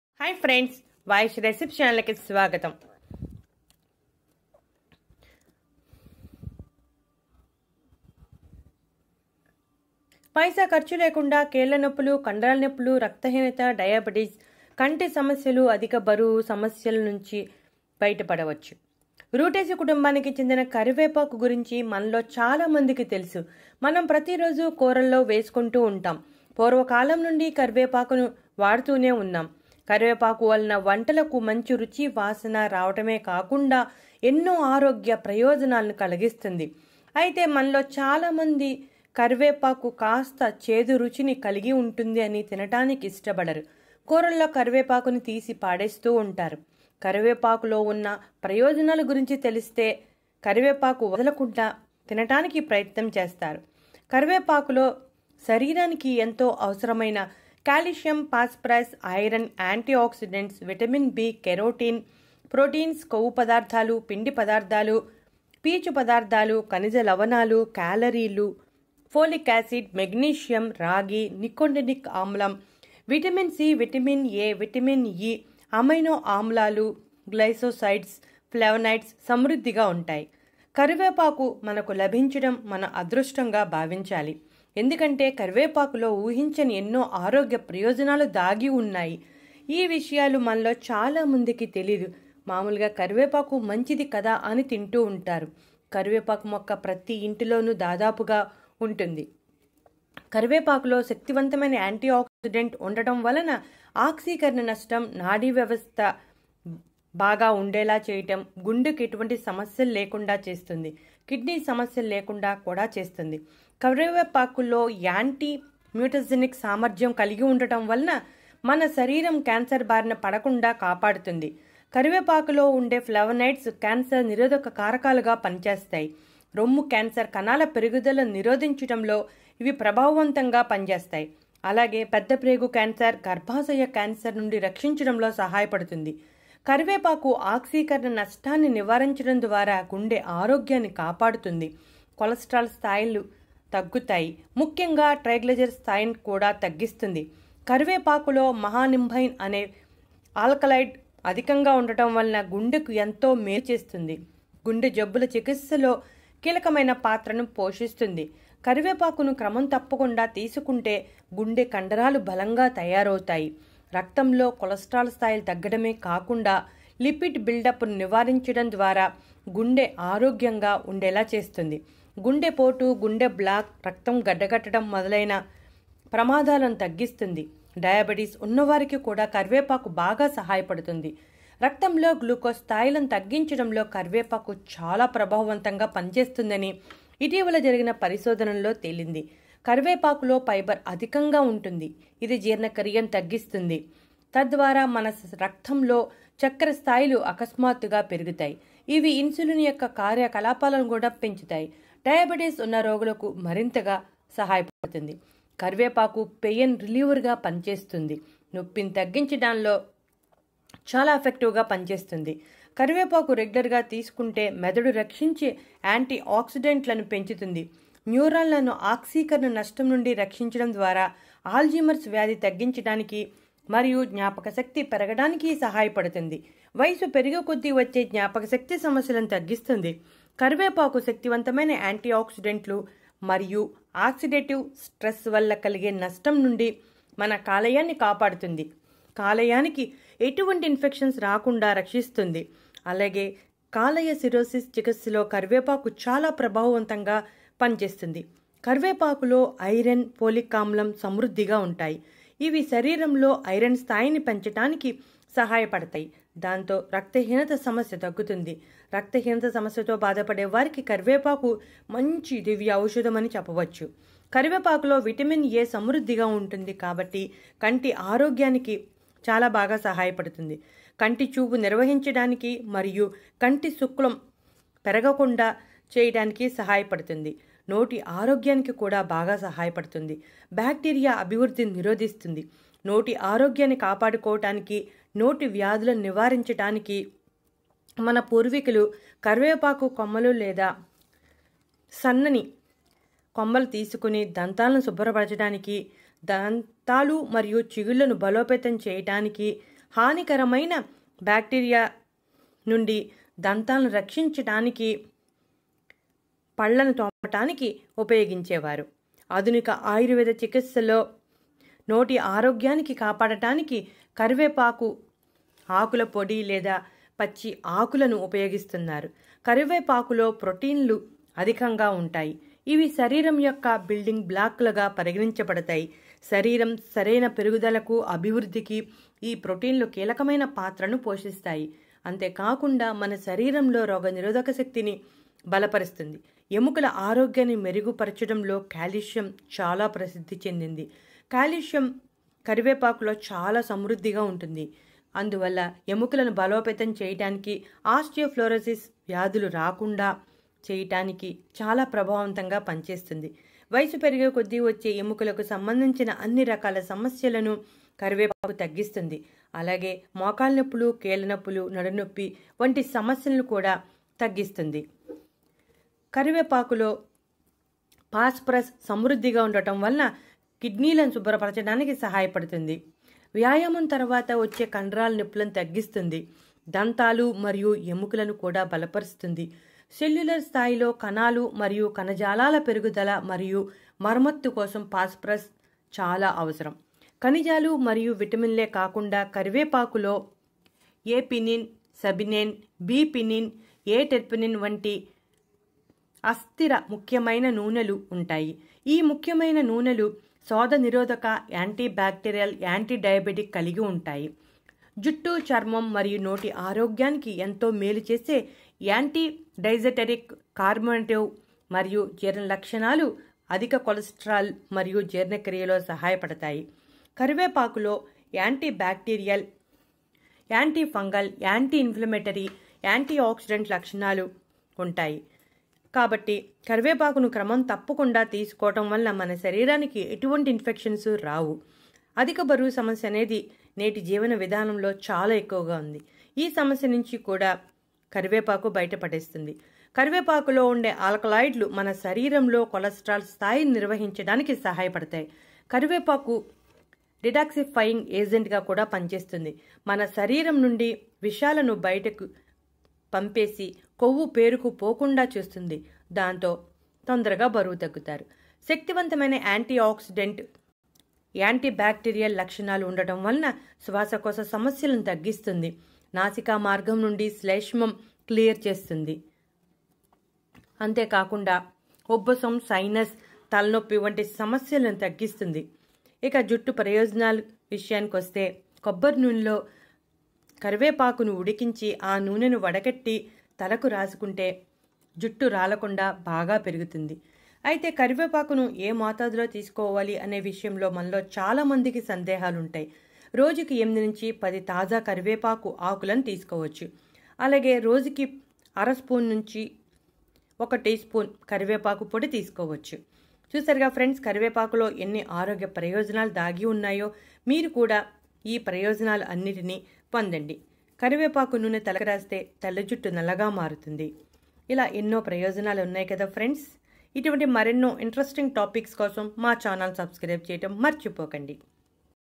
పైసా ఖర్చు లేకుండా కీళ్ల నొప్పులు,కండరాల నొప్పులు,రక్తహీనత,డయాబెటిస్,అధిక బరువు తగ్గించుకోవచ్చు
పైసా ఖర్చు లేకుండా కీళ్ల నొప్పులు,కండరాల నొప్పులు,రక్తహీనత,డయాబెటిస్,కంటి సమస్యలు,అధిక బరువు సమస్యలను తగ్గించుకోవచ్చు